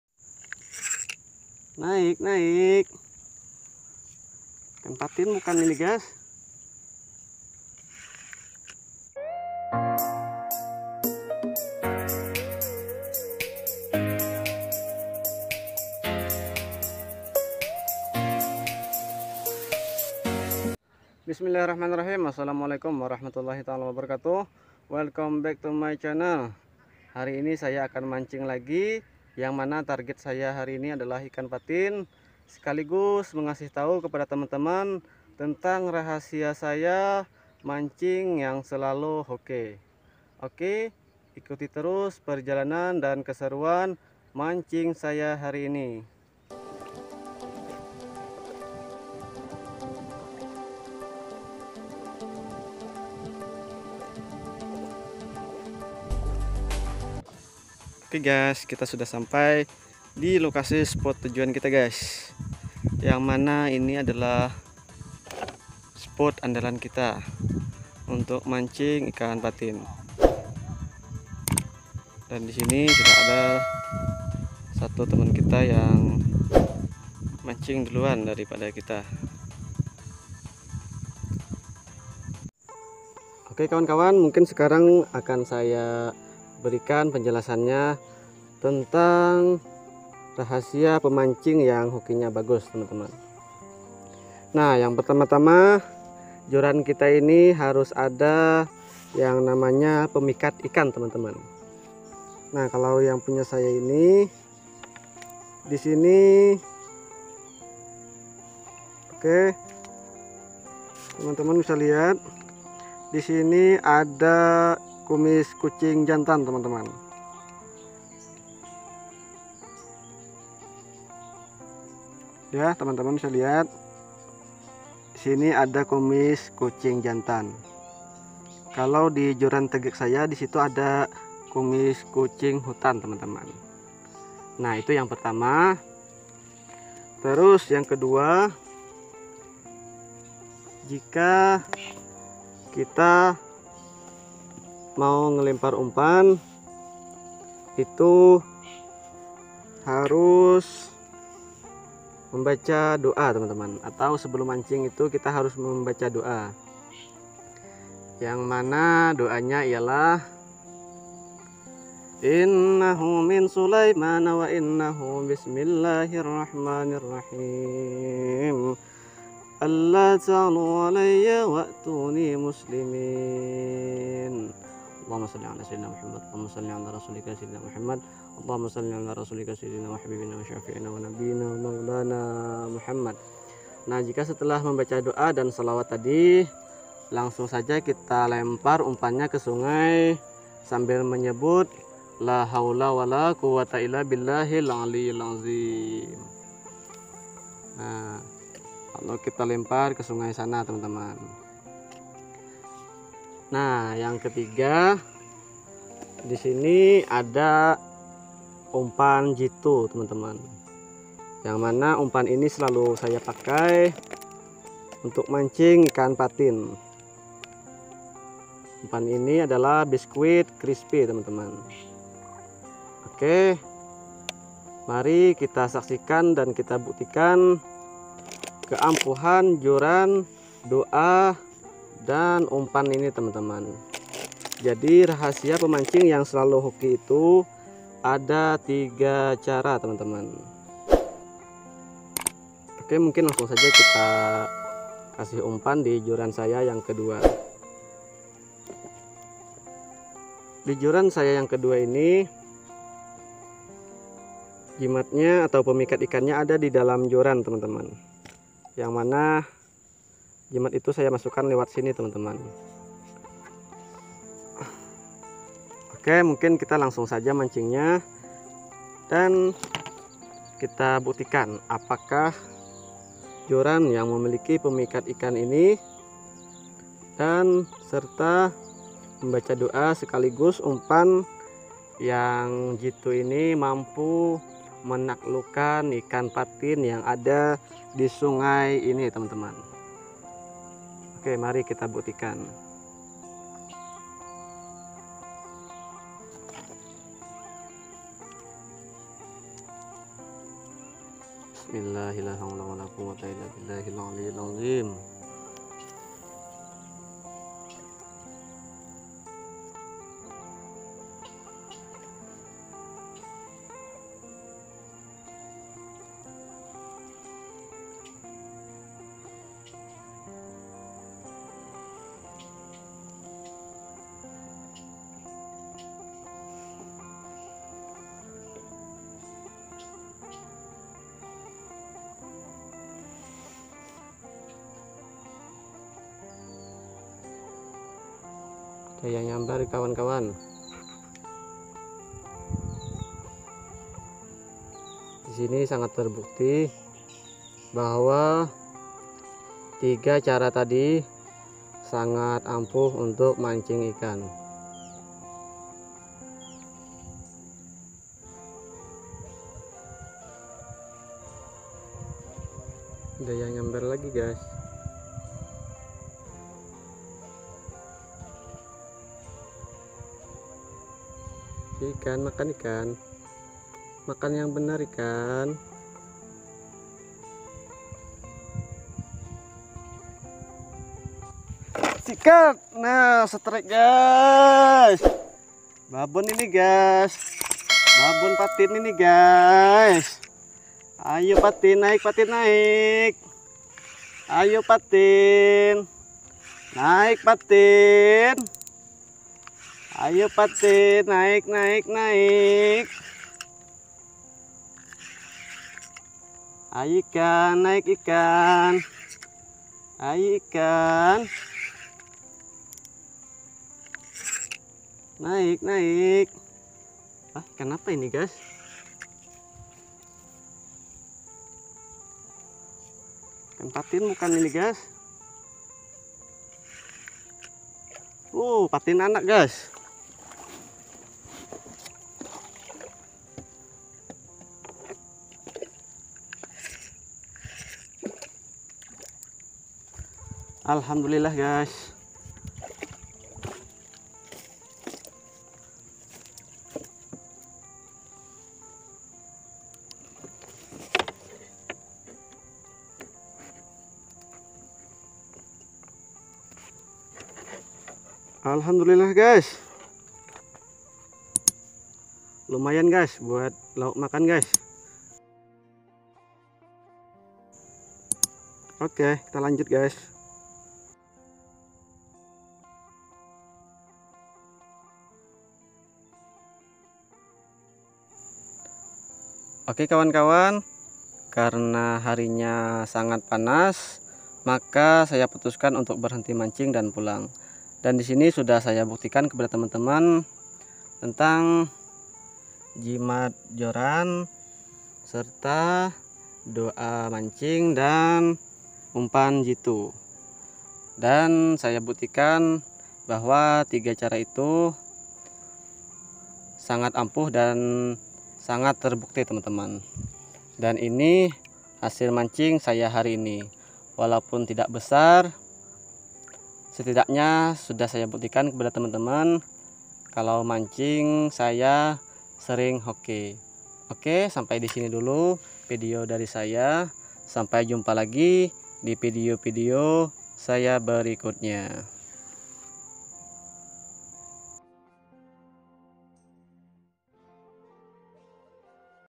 RAHASIA PEMANCING YANG SERING HOKI
video ini menceritakan tentang ;
RAHASIA PEMANCING YANG SERING HOKI, jimat mancing ikan, doa mancing ikan, dan umpan jitu mancing ikan,